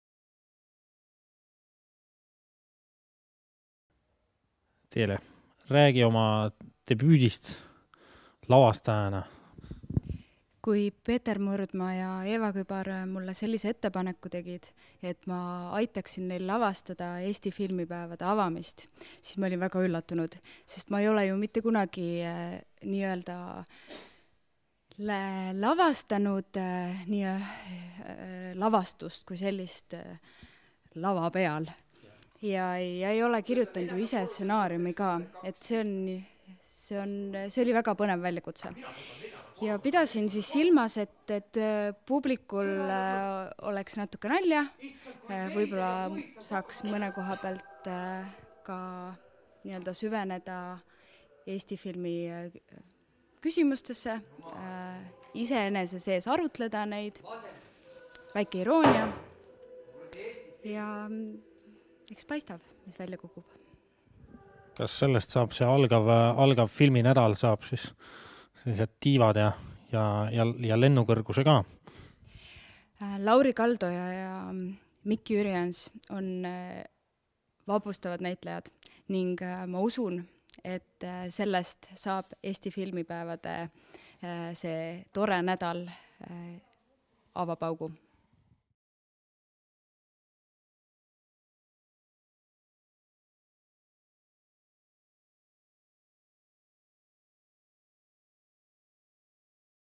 Eesti Filmi Päevad - Filmipäevade avatseremoonia lavastaja Teele Ardel
Telerežissöör Teele Ardel räägib oma esimesest tseremoonialavastusest Eesti Filmi Päevade avamisel.